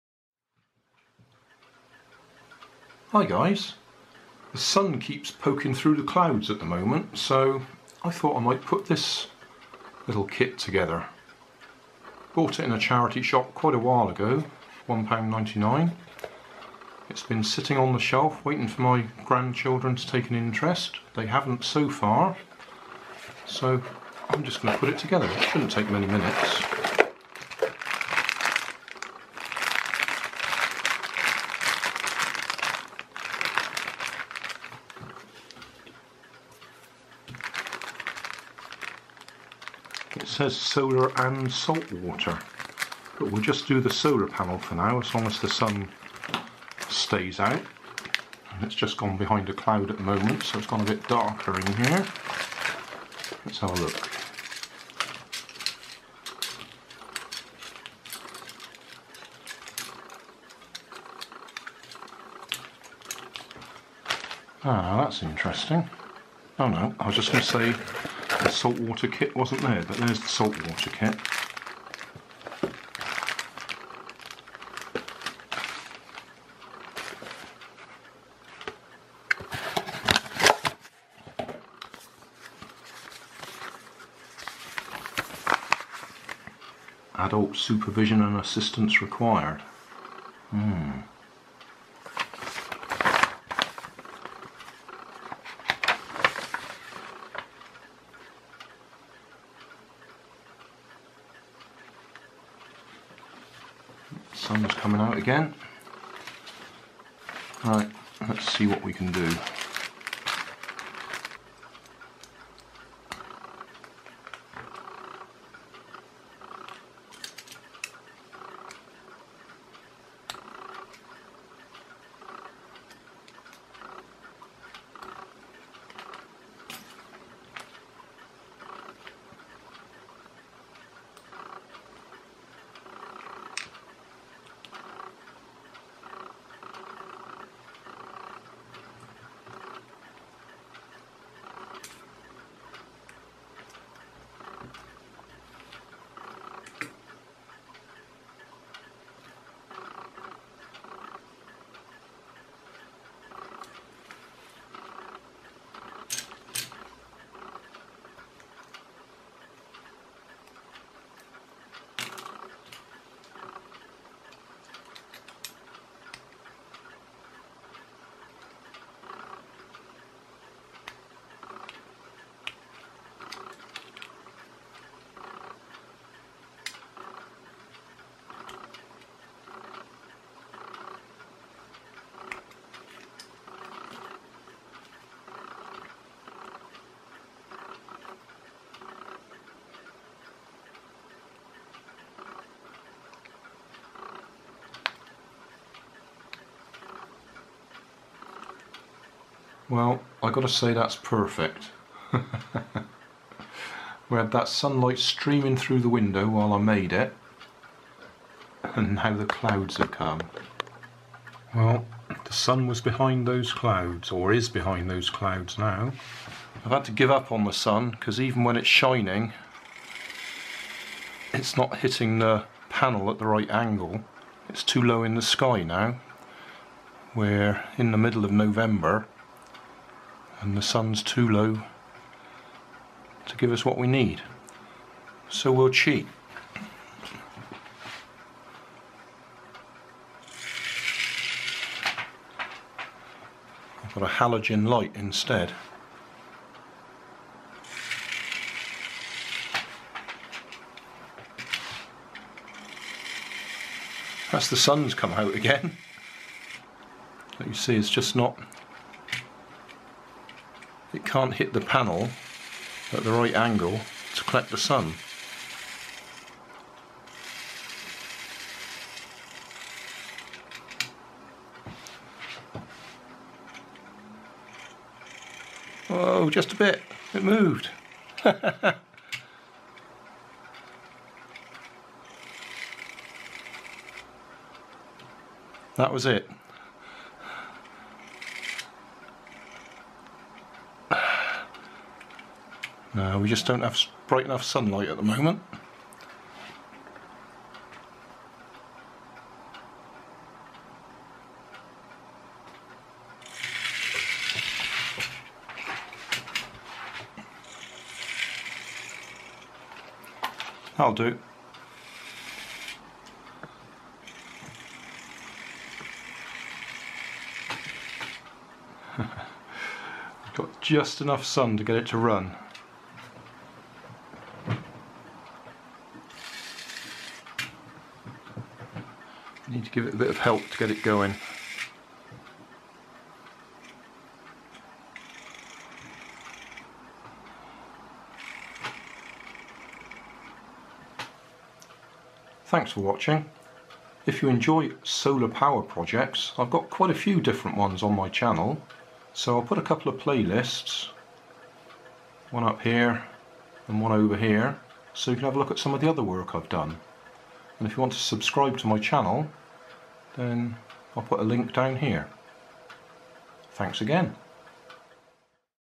Solar and Salt Water Racer - Solar version
Solar and Salt Water Engine Racer Car Kit - Solar Version. I had hoped my grandchildren would assemble this in the sunny weather but they didn't so I did. It is now mid-November and the sun is really too low in the sky to give us the power needed to run this. I do get a short run but I also tested it with a halogen lamp.